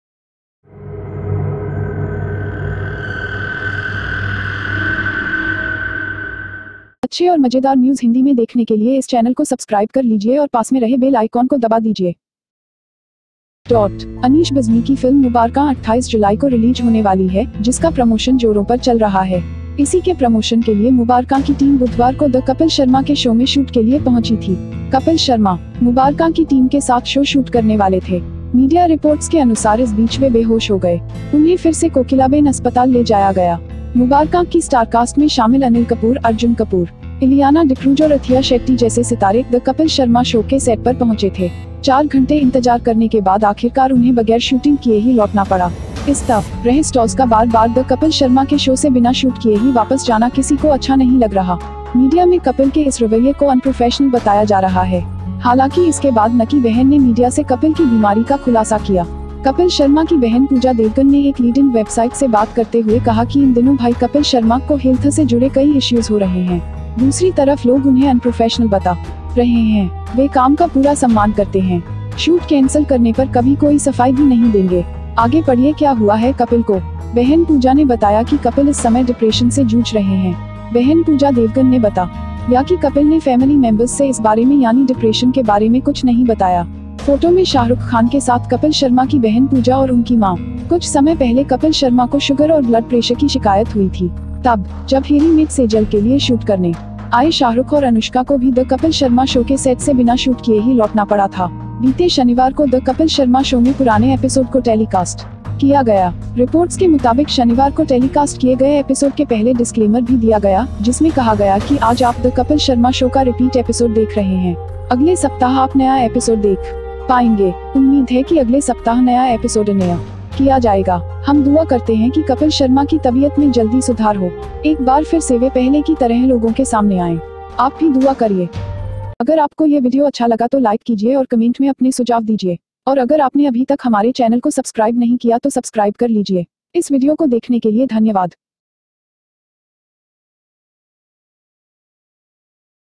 बीमारी से जूझ रहे हैं कपिल, बहन ने किया खुलासा || Bizarre Hindi News
Anneesh Bazmee's movie Mubarak is going to be released on July 28, which is going on in full swing. For the promotion of this, Mubarak's team reached for the shoot at The Kapil Sharma's Show on Wednesday.

Kapil Sharma was the show shooter with Mubarak's team. According to media reports, they became unconscious in the meantime. He was taken to Kokilaben Hospital again.

Anil Kapoor, Arjun Kapoor, Ileana D'Cruz and Athiya Shetty, who were included in the 'Mubarakan' Starcast, reached the set of 'The Kapil Sharma Show'. After waiting for four hours, he had to return without shooting at all.

In this way, it is not good to repeat the stars repeatedly without the shoot of Kapil Sharma's show without shooting. This attitude of Kapil in the media is being described as unprofessional. However, after this Naki sister disclosed Kapil's illness to the media.
Kapil Sharma's sister Pooja Devgan, speaking to a leading website, said that these days, brothers (Kapil Sharma) are having many health related issues. People on the other side are telling them unprofessional. They respect the work perfectly. Will not give any clarification on canceling the shoot. "Next, read what happened to Kapil.

Sister Puja told that Kapil is battling depression this time.

Sister Pooja Devgan told that Kapil did not tell anything about the problem of depression from family members about this. (Kapil Sharma's sister Puja and her mother along with Shahrukh Khan in the photo)

Some time ago Kapil Sharma had a complaint of sugar and blood pressure. Then, when Shahrukh and Anushka, who came to shoot for "When Mera Mate Sejal", had to return without shooting without the set of The Kapil Sharma Show.

The past episodes were telecasted on Saturday at The Kapil Sharma Show. According to the reports, on Saturday, before the telecasted episodes, a discclaimer was also given, which stated that "Today you are watching the repeat episode of The Kapil Sharma show ... Next week you will be able to watch new episodes, hopefully next week new episodes Will be annoyed.

We pray that Kapil Sharma's health will improve quickly. Once again they come in front of people like before. You too pray

सुक्रिया,धन्यवाद,सबाखैर.